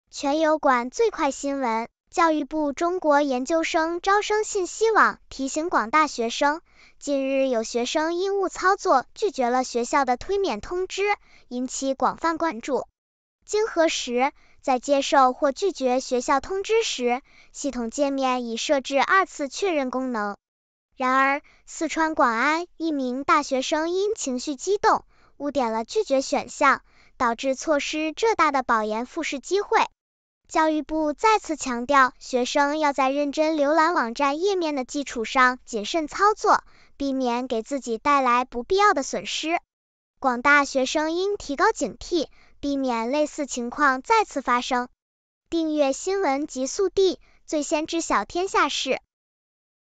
中国新闻10月03日09时：研招网回应女生手滑拒绝保研通知：系统界面均设计有二次确认功能
教育部中国研究生招生信息网提醒广大学生，近日有学生因误操作拒绝了学校的推免通知，引起广泛关注。经核实，在接受或拒绝学校通知时，系统界面已设置二次确认功能。然而，四川广安一名大学生因情绪激动，误点了“拒绝”选项，导致错失浙大的保研复试机会。教育部再次强调学生要在认真浏览网站页面的基础上谨慎操作，避免给自己带来不必要的损失。广大学生应提高警惕，避免类似情况再次发生。